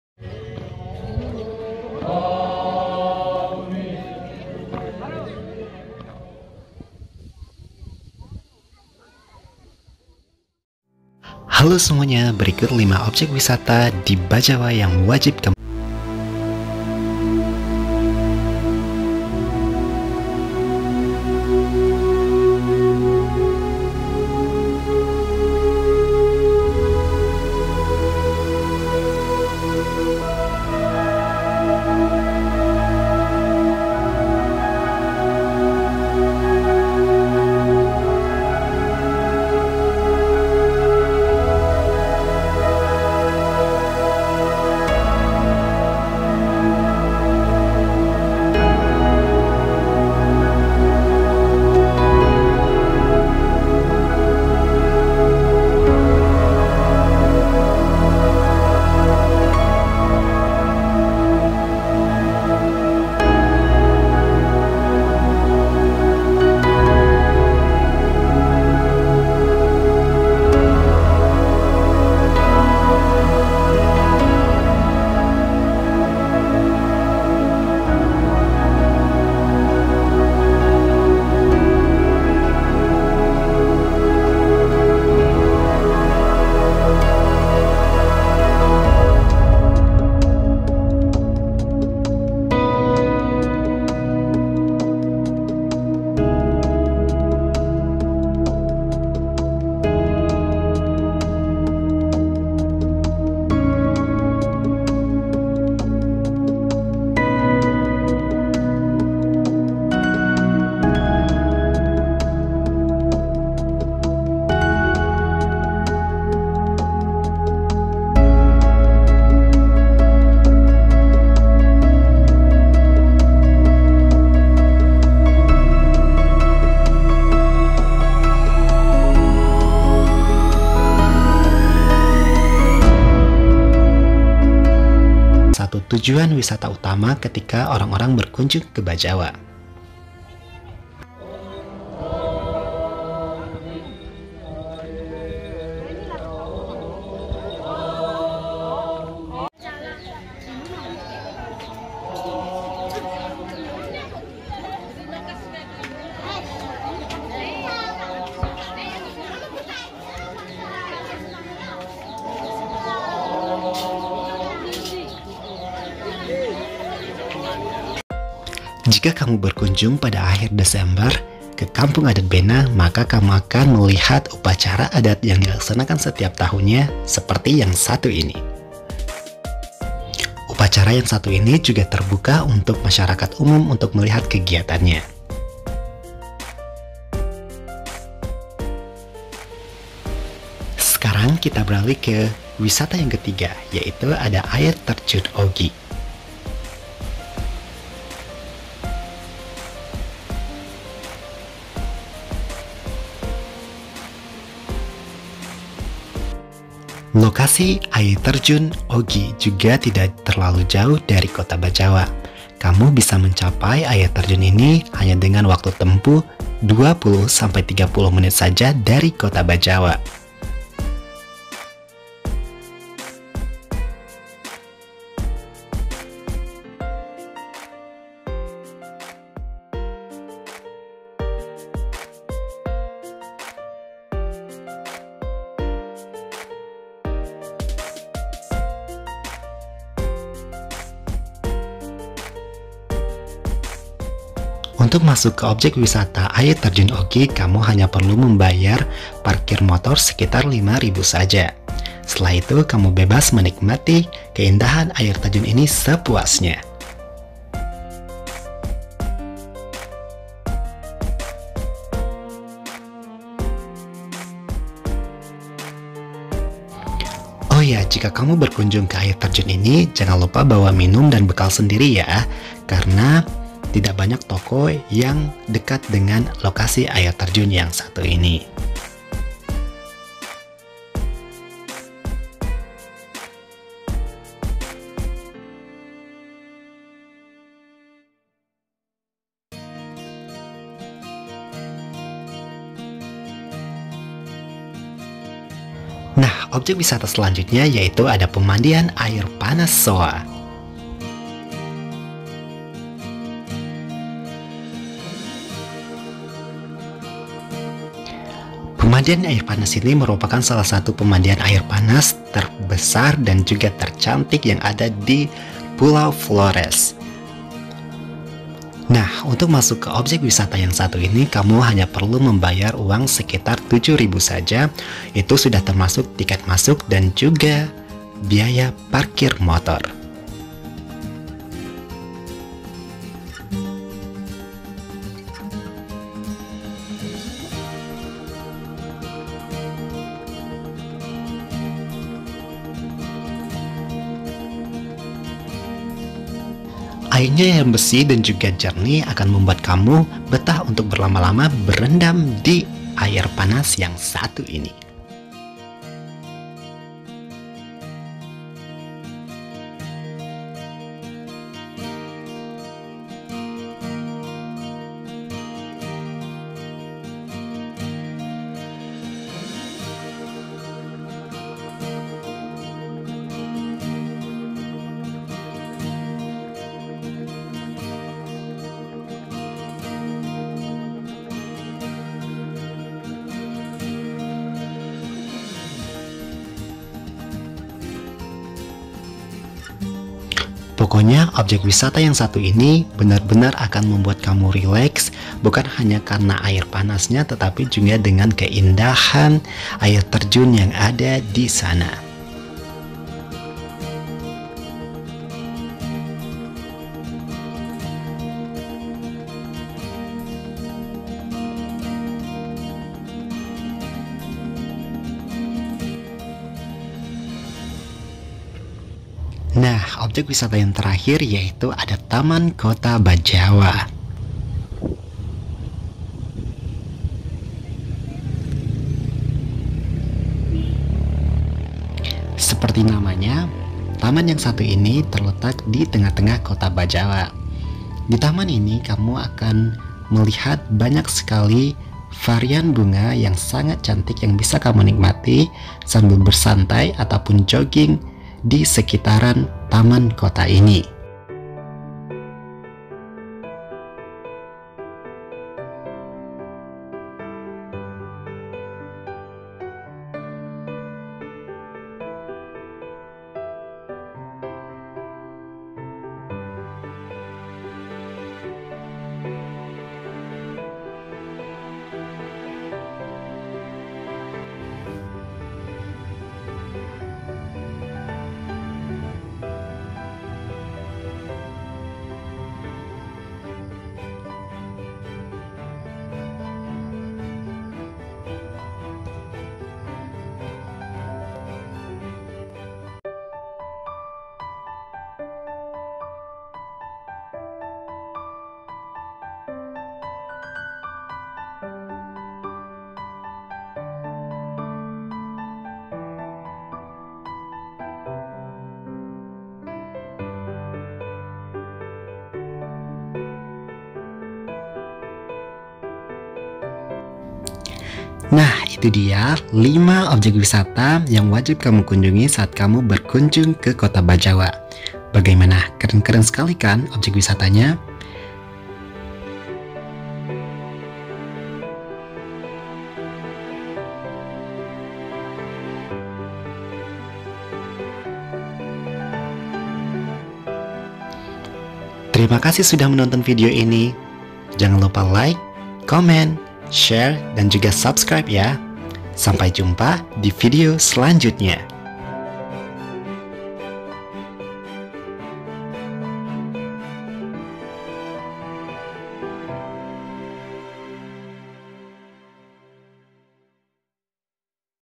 5 Rekomendasi Objek Wisata Di Bajawa, Flores, NTT.
Halo  Semuanya, Jika  kalian ingin berwisata ke Bajawa, yuk check 5 rekomendasi wisata yang ada di video ini !

1. Bukit Wolobobo
2.Kampung Adat Bena
3. Air Tejun Ogi
4. Air Panas So'a
5. Taman Kota Bajawa

Selamat Berwisata, and keep the place clean!!